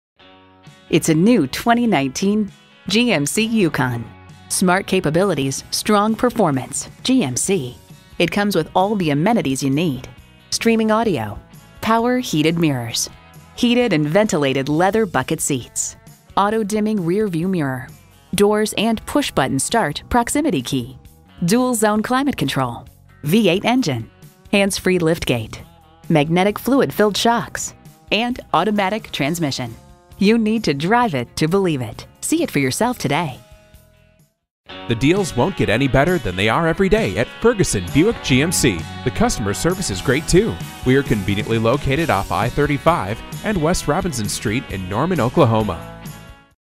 New 2019 GMC YUKON DENALI Norman OK Oklahoma-City, OK
Year: 2019
Make: GMC
Model: YUKON DENALI
Engine: 6.2 Liter 8 Cylinder
Transmission: 6 Speed Automatic
Color: Satin Steel Metallic
Interior: Cocoa/Dark Atmosphere
Stock #: G1391
VIN: 1GKS2CKJ1KR191036

Address:
1015 North Interstate Drive